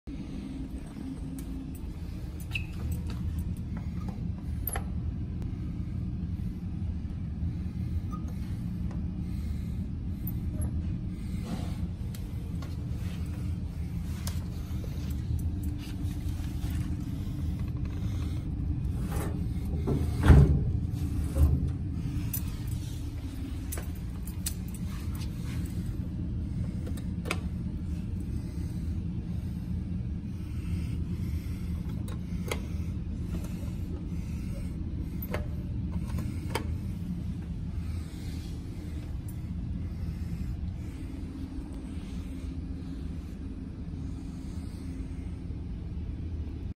Tucson Elevator Co Traction Dumbwaiter @ Chemistry Building UA Tucson AZ (no voiceover)
I FOUND MY FIRST DUMBWAITER! 😃😃😃😃😃😃😃😃😃

Located in the chemistry building in the u of a